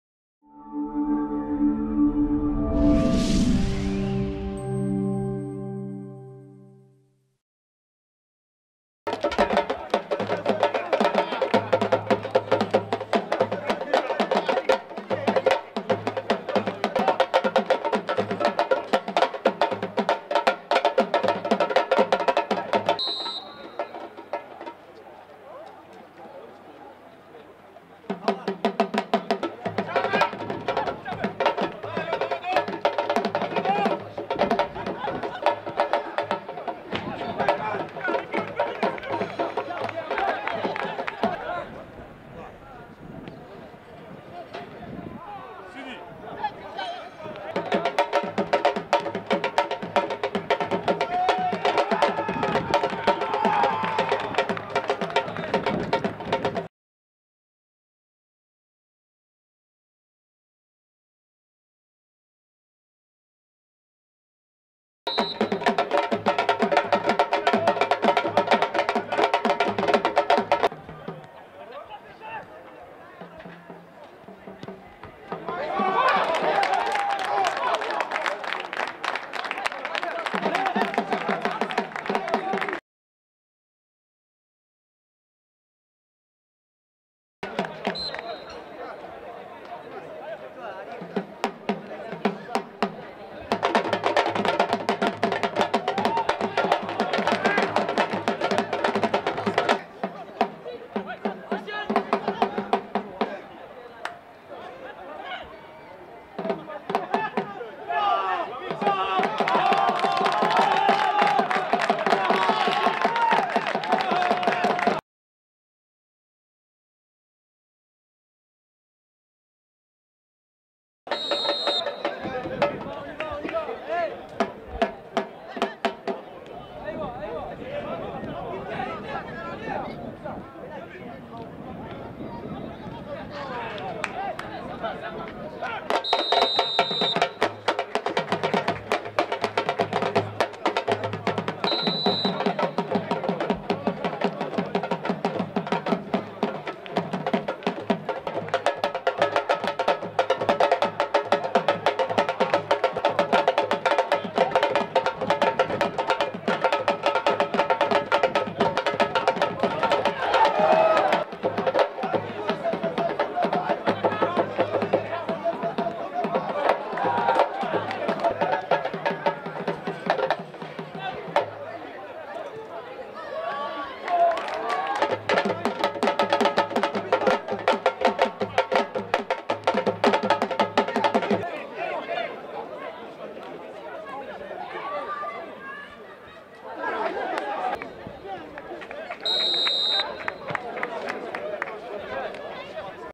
J6 - Super D1: Nouakchott King's - AS Police (2-1), le résumé
Retrouvez les temps forts de la rencontre Nouakchott King's - AS Police, comptant pour la sixième journée du Championnat National Super D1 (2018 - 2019)